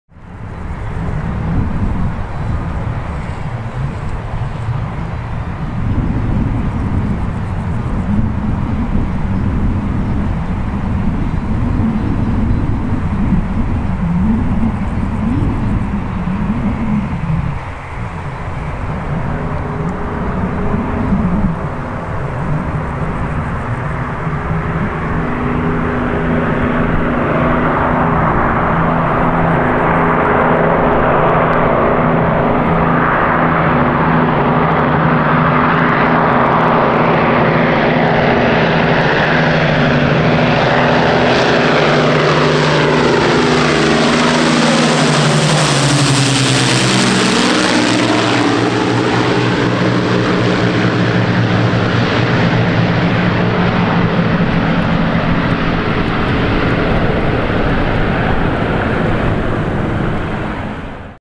Lancaster bomber low overhead | Beautiful sound
What's better than the sound of a Rolls-Royce Merlin engine? Four of them in close unison!

Here is Avro Lancaster PA474, one of only two airworthy examples remaining in the world, landing after an air show. I'll let the merlins do the talking...